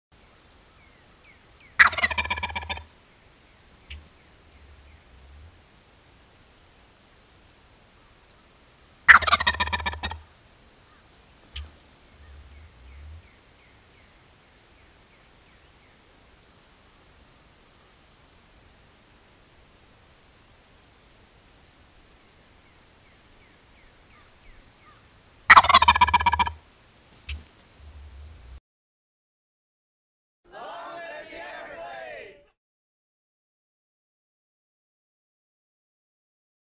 Male Osceola Turkey Gobbles on Camera!
Did you know that male turkeys are called "Gobblers" due to the vocalizations that they make?

Tune in for more Everglades Education with ALLIGATOR RON BERGERON!